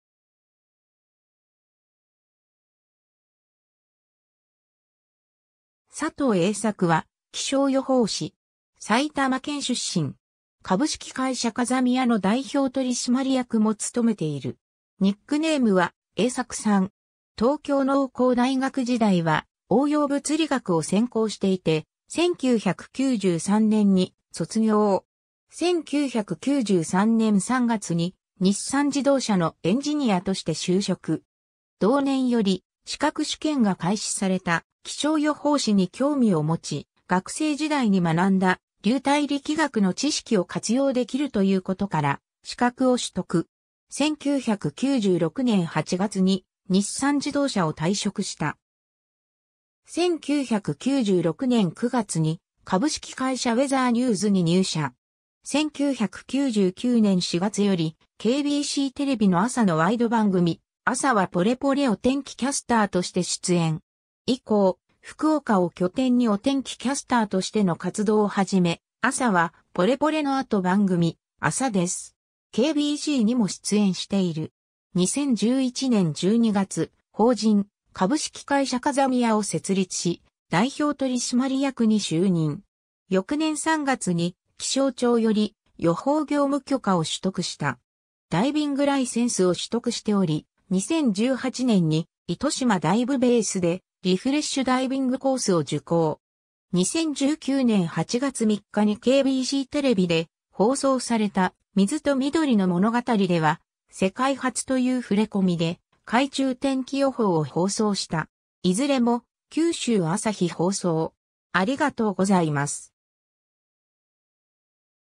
佐藤栄作 (気象予報士)
佐藤 栄作（さとう えいさく） は、気象予報士。
埼玉県出身。
株式会社 風見屋の代表取締役も務めている。
 ニックネームは「栄作さん」。
 東京農工大学時代は応用物理学を専攻していて、1993年に卒業。
 1993年3月に日産自動車のエンジニアとして就職。
同年より資格試験が開始された気象予報士に興味を持ち、学生時代に学んだ流体力学の知識を活用できるということから資格を取得。
1996年8月に日産自動車を退職した。
 1996年9月に株式会社ウェザーニューズに入社。
 1999年4月よりKBCテレビの朝のワイド番組『朝はポレポレ』お天気キャスターとして出演。
以降、福岡を拠点にお天気キャスターとしての活動をはじめ、『朝はポレポレ』の後番組『アサデス。
KBC』にも出演している。
 2011年12月、法人「株式会社 風見屋」を設立し、代表取締役に就任。
翌年3月に気象庁より予報業務許可を取得した。
 ダイビングライセンスを取得しており、2018年に糸島ダイブベースでリフレッシュダイビングコースを受講。
2019年8月3日にKBCテレビで放送された『水と緑の物語』では、世界初という触れ込みで、海中天気予報（海に潜ったまま天気予報を中継）を放送した。
 いずれも九州朝日放送